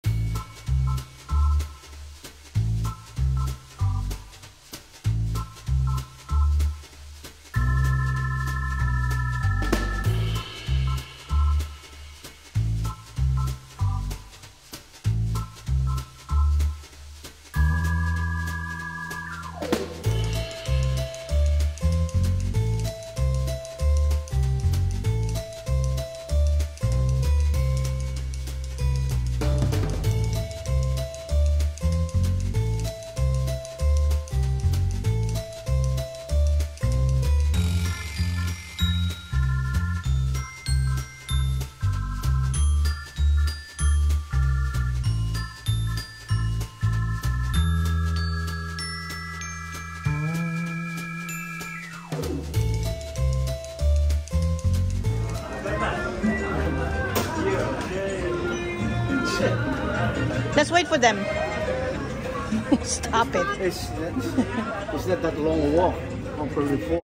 From Rome to Ravenna
We flew from JFK to Rome then took the train to Ravenna where our ship will be for our 7 nights cruise to Croatia and Greece.